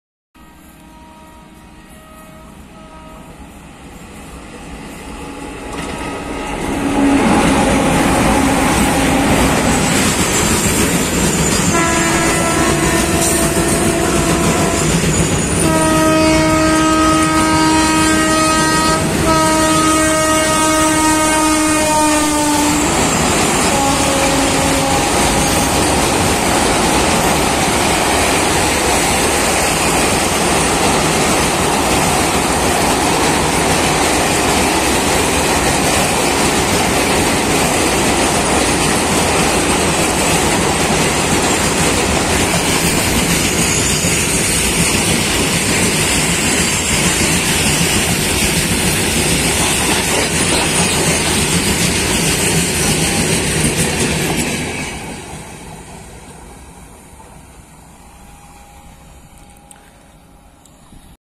Terrific Parallel Overtaking | Speedy Wap7 Raceing Twins Wag5 Over The Bridge || Incretebel India
Excilent Parallel Overtaking Train Incredible India